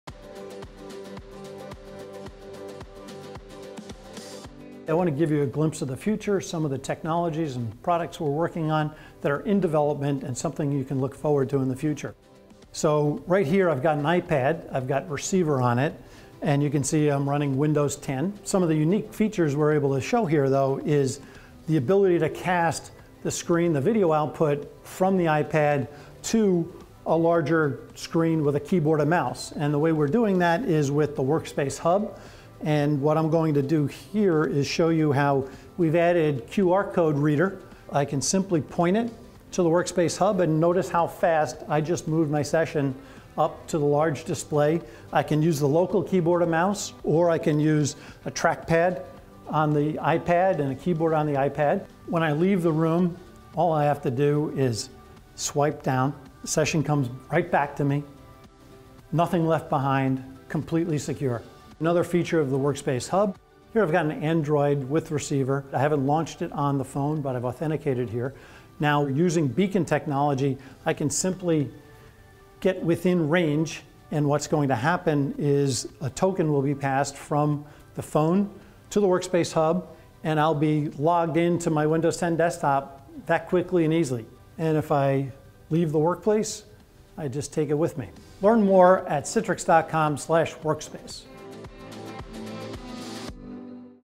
Citrix Workspace Hub – Powering the Future of Work
What does the future of work look like? Screen casting from tablets to monitors, proximity login and more with Citrix Workspace Hub keep workers productive while they are on-the-go.